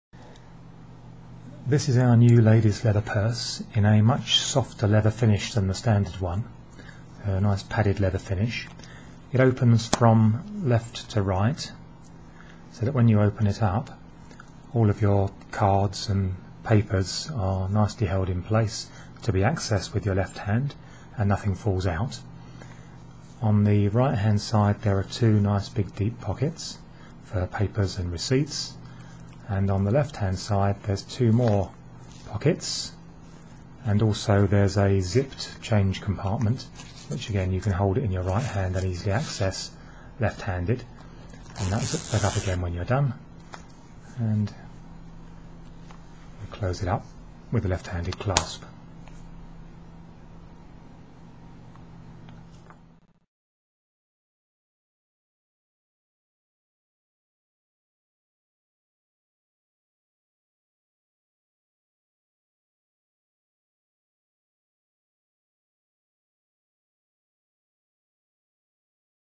Ladies purse left handed in soft leather
- Left handed ladies purse in soft leather with fully reversed design so nothing falls our when you open it from left to right